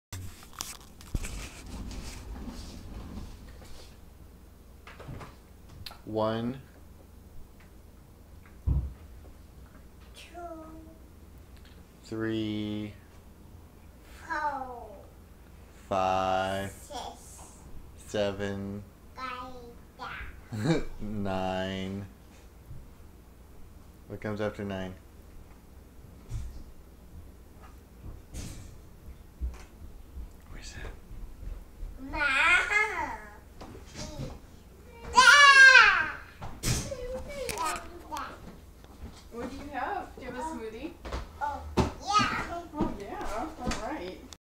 Sort of counting, 21 months
My daughter sort of reciting numbers. Sometimes she gets all the way through to 11